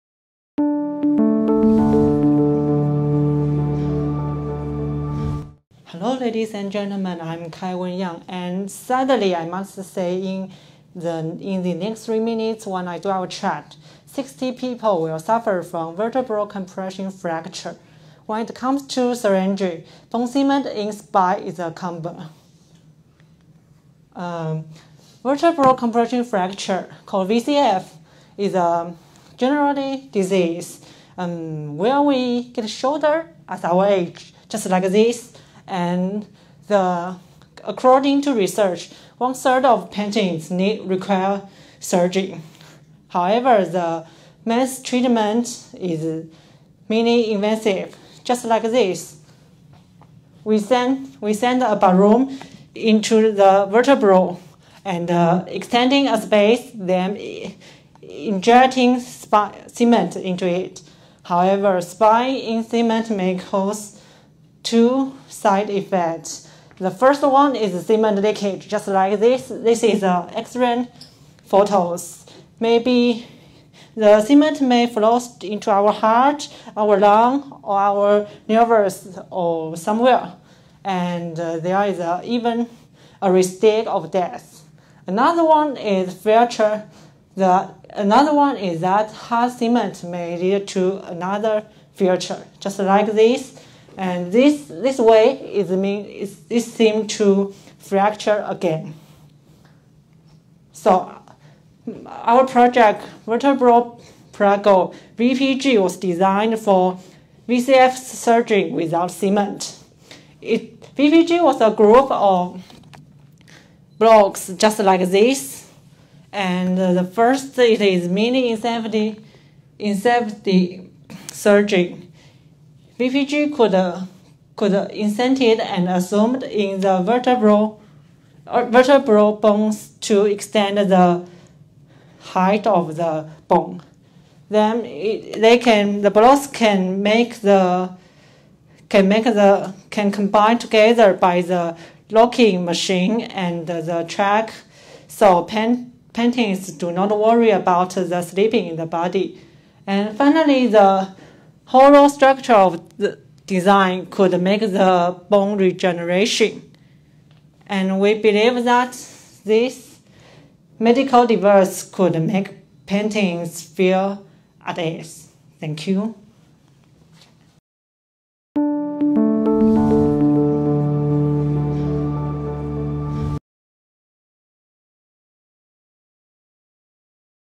2021 Falling Walls Lab Taipei - 1st Place
2021 Falling Walls Lab Taipei - 1st Place (第一名)
Name: KAI-WEN YANG
Title: BREAKING THE WALL OF CEMENTING IN SPINE
National Taiwan University